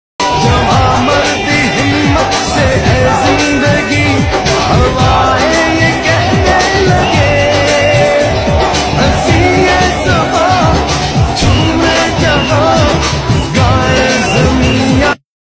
میچ جیتنے کے بعد پاکستانیوں کا جشن ❤
Pakistan & Afghanistan cricket world cup 2019 match winning team